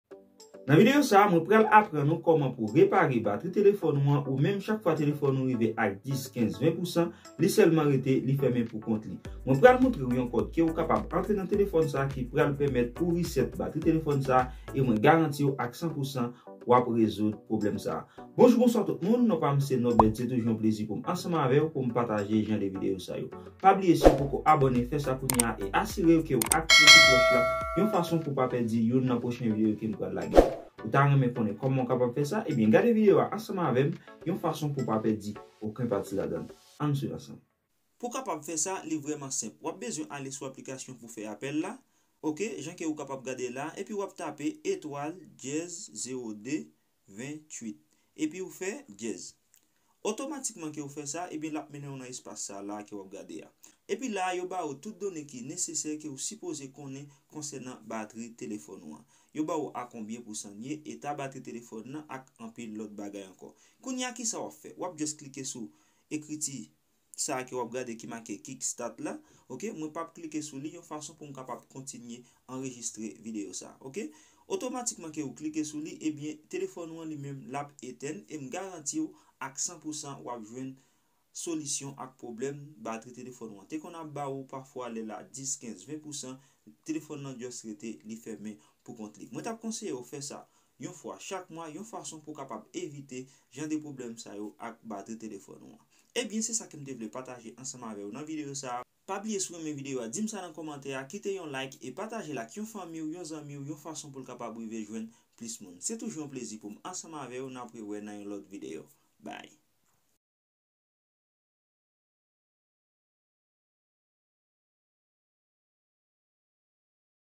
PWOBLEM BATRI TELEPHONE RESET BATRI TELEPHONE OU
PWOBLEM BATRI TELEPHONE Ou pral aprann kòman ou kapab repare batri telephone poul kapab kenbe chage#RESET BATRI TELEPHONE OU